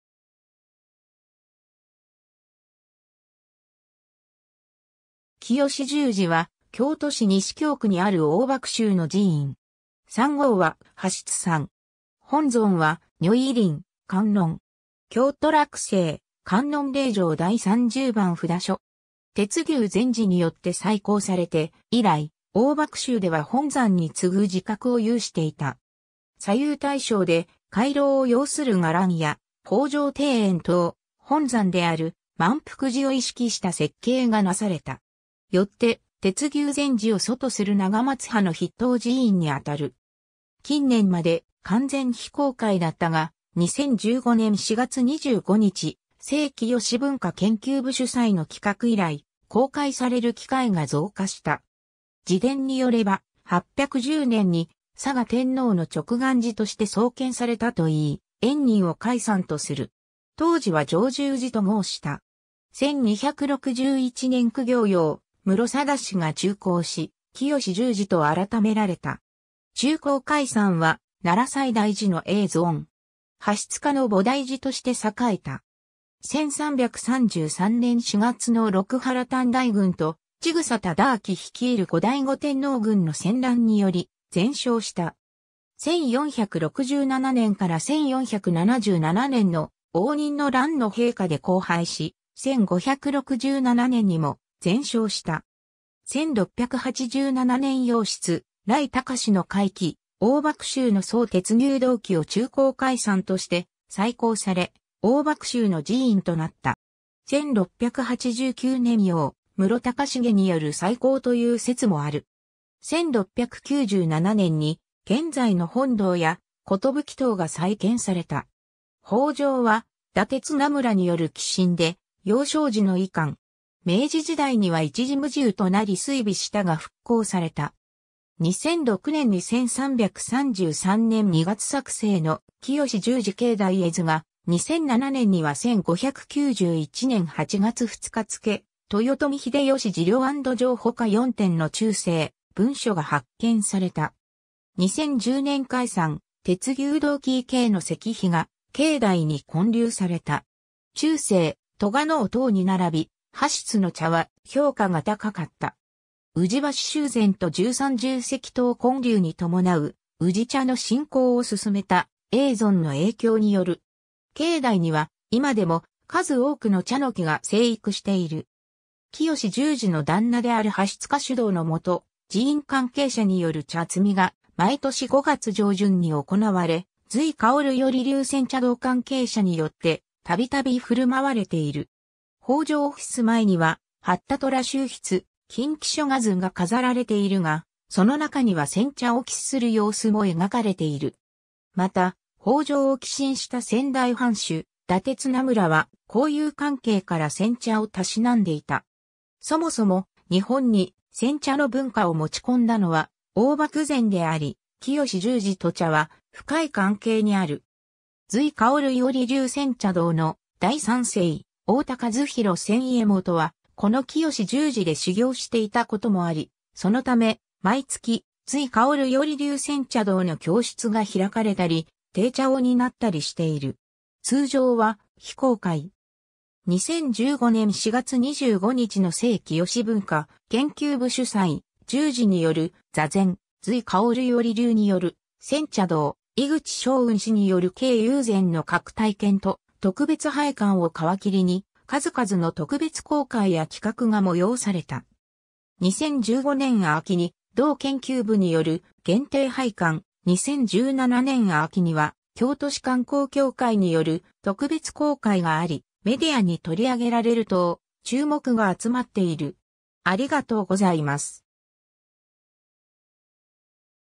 浄住寺
浄住寺（じょうじゅうじ）は、京都市西京区にある黄檗宗の寺院。
山号は葉室山。
本尊は如意輪観音。
京都洛西観音霊場第三十番札所。
 鉄牛禅師によって再興されて以来、黄檗宗では本山に次ぐ寺格を有していた。
左右対称で回廊を擁する伽藍や、方丈庭園等、本山である萬福寺を意識した設計がなされた。
よって、鉄牛禅師を祖とする長松派（萬福寺塔頭長松院）の筆頭寺院にあたる。
 近年まで完全非公開だったが、2015年4月25日、精浄文化研究部主催の企画以来、公開される機会が増加した。
 寺伝によれば、810年（大同5年、弘仁元年）に嵯峨天皇の勅願寺として創建されたといい、円仁（慈覚大師）を開山とする。
当時は常住寺と号した。
 1261年（弘長元年）公卿葉室定嗣が中興し、浄住寺と改められた。
中興開山は奈良西大寺の叡尊。
葉室家の菩提寺として栄えた。
 1333年（正慶2年）4月の六波羅探題軍と千種忠顕率いる後醍醐天皇軍の戦乱により全焼した。
 1467年（応仁元年）から1477年（文明9年）の応仁の乱の兵火で荒廃し、1567年（永禄10年）にも全焼した。
 1687年（貞享4年）葉室頼孝の開基、黄檗宗の僧鉄牛道機を中興開山として再興され、黄檗宗の寺院となった。
 1689年（元禄2年）葉室孝重による再興という説もある。
 1697年（元禄10年）に現在の本堂や寿塔が再建された。
 方丈は伊達綱村による寄進で、幼少時の遺館。
 明治時代には一時無住となり衰微したが復興された。
 2006年（平成18年）に1333年（正慶2年）2月作成の浄住寺境内絵図が、 2007年（平成19年）には1591年（天正19年）8月2日付豊臣秀吉寺領安堵状ほか4点の中世文書が発見された。
 2010年（平成22年）開山鉄牛道機遺掲の石碑が境内に建立された。
 中世、栂尾等に並び、葉室の茶は評価が高かった。
宇治橋修繕と十三重石塔建立に伴う、宇治茶の振興をすすめた叡尊の影響による。
境内には今でも数多くの茶ノ木が生育している。
浄住寺の檀那である葉室家主導のもと、寺院関係者による茶摘みが毎年5月上旬に行われ、瑞芳菴流煎茶道関係者によって度々振る舞われている。
 方丈襖絵には八田虎州筆『琴棋書画図』が飾られているが、その中には煎茶を喫する様子も描かれている。
また、方丈を寄進した仙台藩主伊達綱村は、交友関係から煎茶を嗜んでいた。
そもそも、日本に煎茶の文化を持ち込んだのは黄檗禅であり、浄住寺と茶は深い関係にある。
瑞芳菴流煎茶道の第三世・大田和博仙家元は、この浄住寺で修行していたこともあり、そのため毎月瑞芳菴流煎茶道の教室が開かれたり、呈茶を担ったりしている。
 通常は非公開。
2015年4月25日の精浄文化研究部主催、住持による坐禅、瑞芳菴流による煎茶道、井口翔雲氏による京友禅の各体験と特別拝観を皮切りに、数々の特別公開や企画が催された。
2015年秋に同研究部による限定拝観、2017年秋には京都市観光協会による特別公開があり、メディアに取り上げられる等、注目が集まっている。